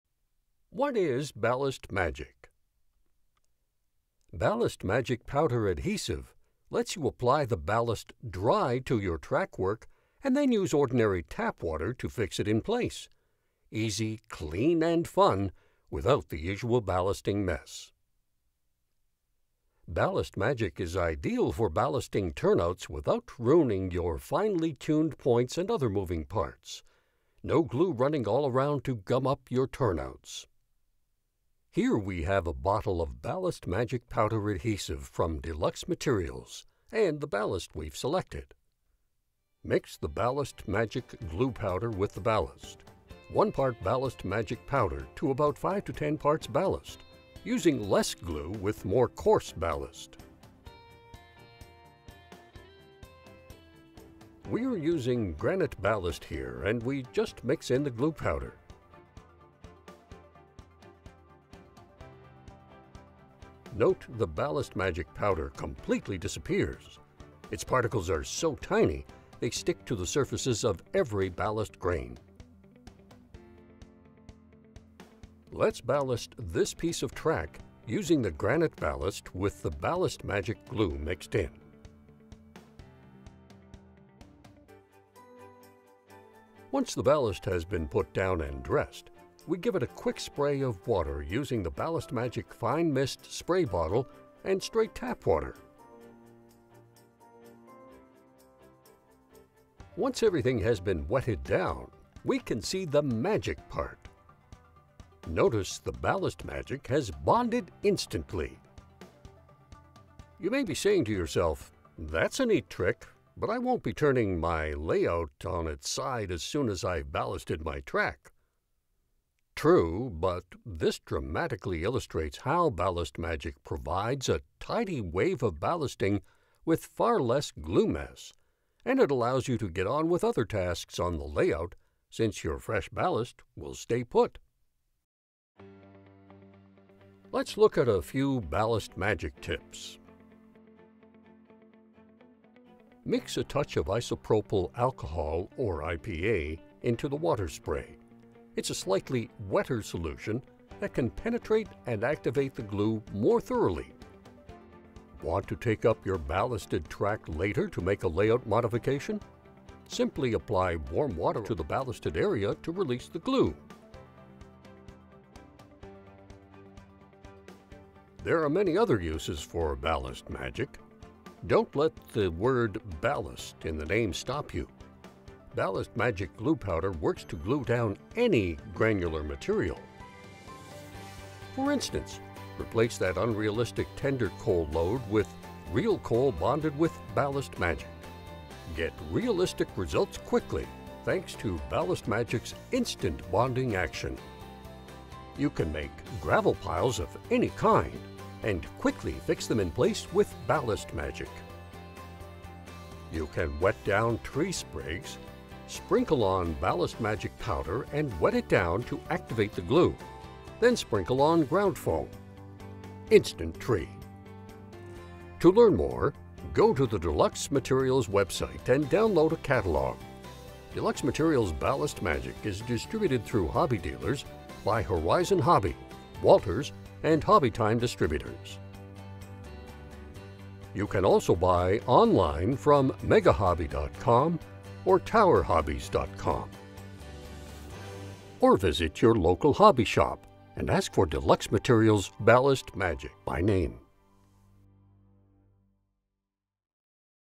Goodbye to the ballasting mess - Deluxe Materials BALLAST MAGIC
Wouldn't it be wonderful if you could ballast turnouts and not gum everything up? Turns out there is just such a product! This 4 min video tells you all about it.

▼ CONNECT!